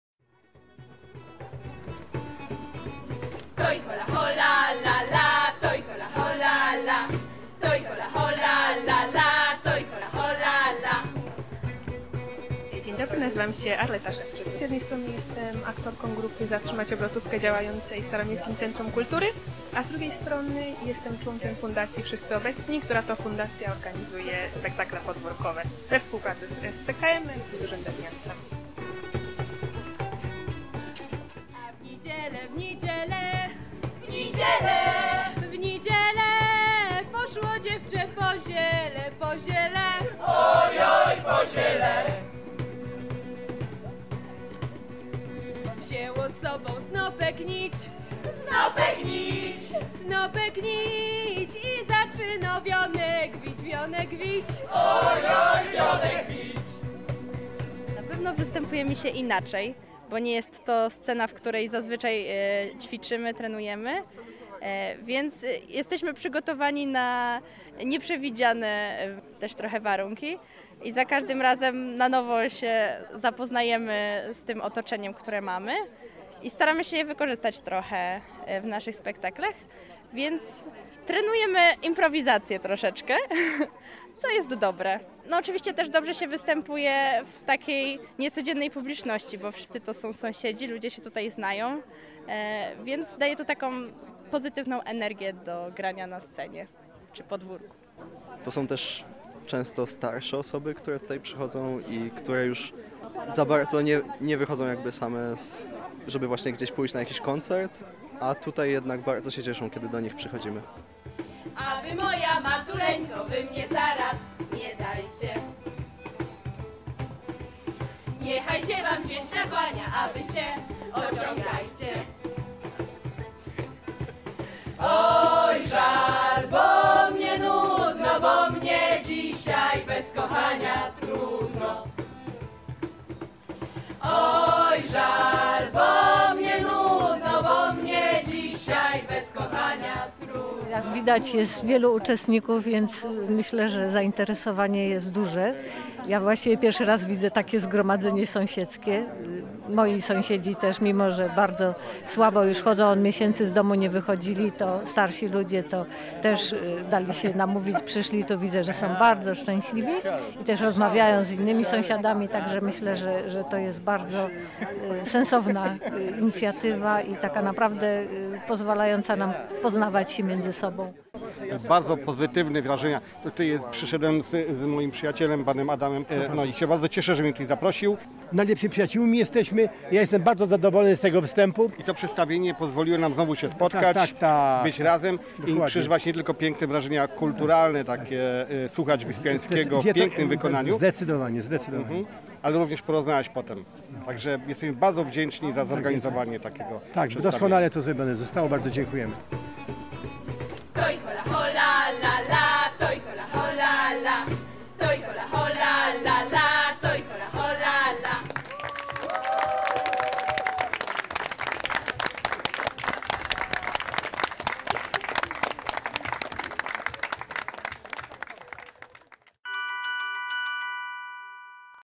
Podwórka żyją Wyspiańskim
We wrześniu 2019 roku na podwórkach kamienic przy ulicy Wietora w Krakowie miało miejsce wydarzenie taneczno-muzyczno wydarzenie "Podwórka żyją Wyspiańskim". Młodzi artyści wyszli ze swoją sztuką do ludzi, którzy sami z powodu wieku czy stanu zdrowia nie wychodzą ze swoich mieszkań. Dzięki pokazowi w kamienicach widownią były okna i balkony.
"Podwórka żyją Wyspiańskim" to wspólne dzieło Staromiejskiego Centrum Kultury Młodzieży i Fundacji Wszyscy Obecni. Zapraszamy na relację z tego wydarzenia!